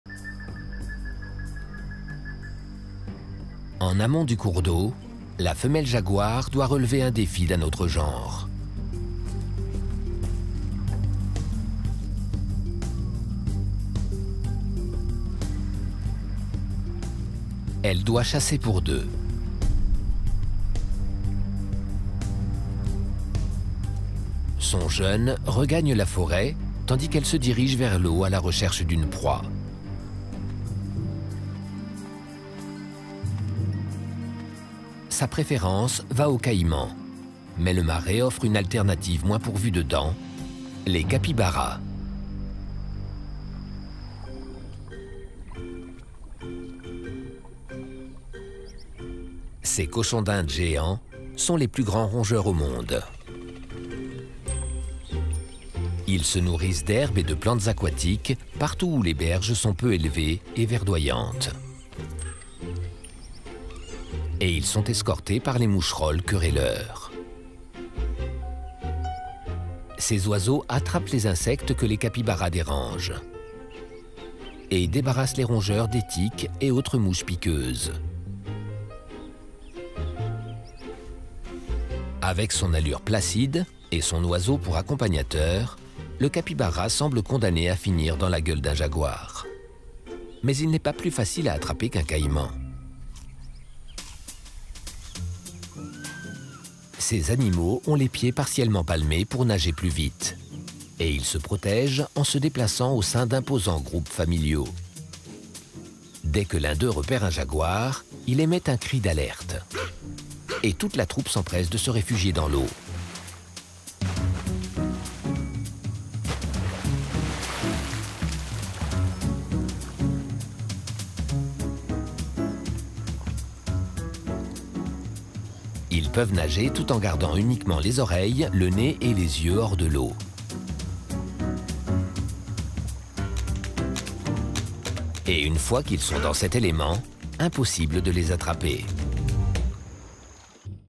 Le capybara, une proie facile pour le jaguar ?
S'il n'est pas aussi dangereux qu'un caïman, le plus grand rongeur du monde peut compter sur sa vie en groupe et son aisance dans l'eau pour se sauver des griffes des prédateurs terrestres.